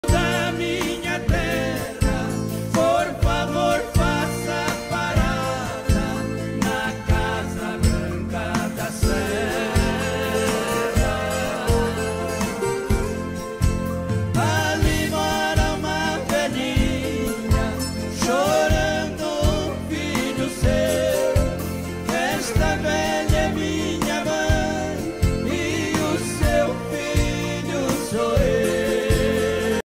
Petra EJF - égua mangalarga marchador pampa de preto a venda
N1cavalos venda de égua mangalarga marchador filha de CARTUCHO DA COCHEIRA.